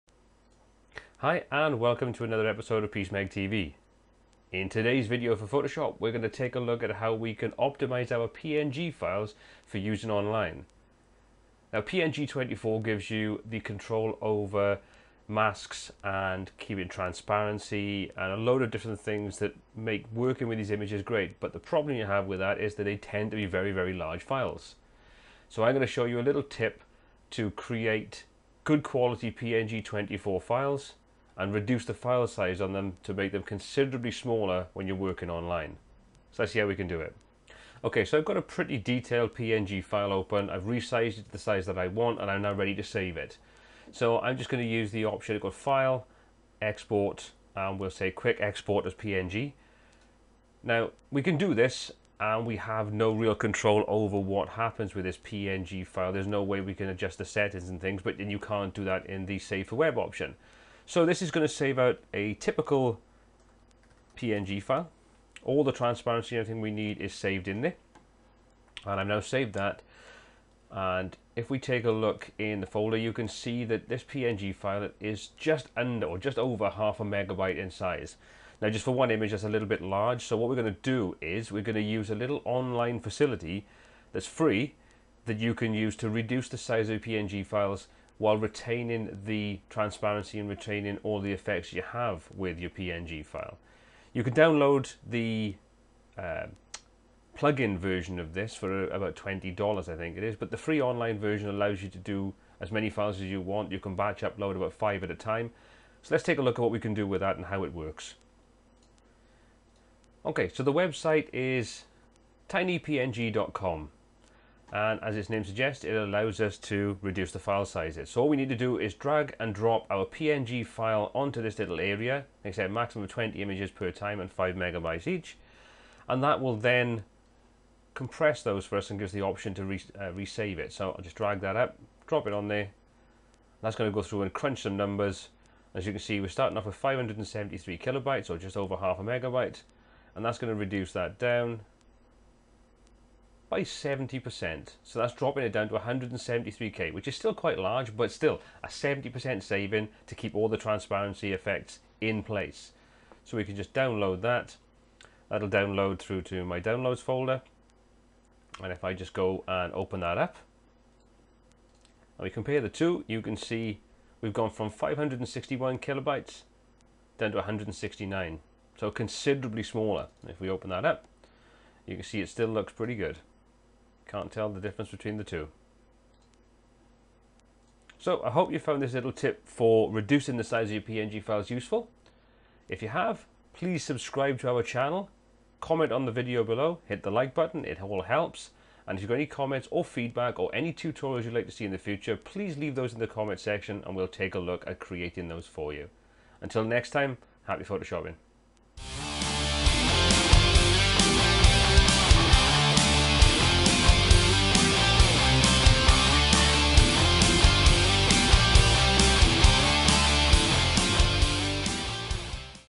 Photoshop Tutorial: Mega Shrink your PNG Images
Crunch Your PNGs! This technique will help you reduce your 24 bit PNG images by up to 90% in size!

Follow this simple technique to help optimize your images for the Internet while keeping all the quality and transparency benefits of the PNG file format!